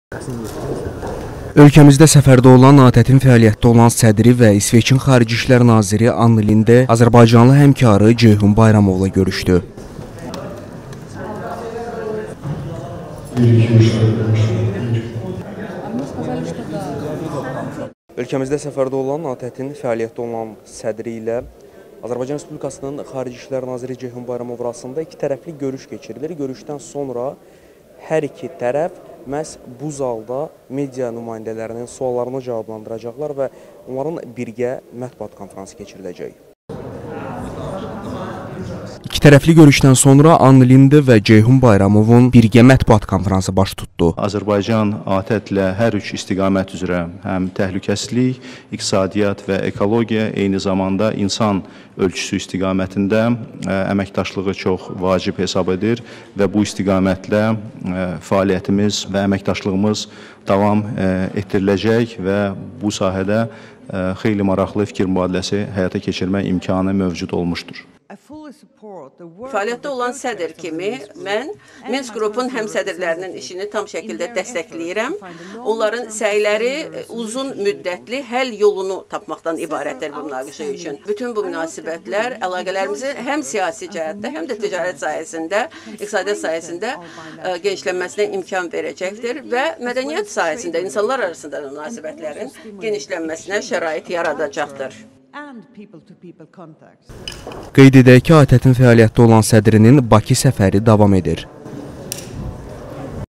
Ann Linde ilə Ceyhun Bayramovun birgə mətbuat konfransı keçirilib
ATƏT sədri Bakıdadır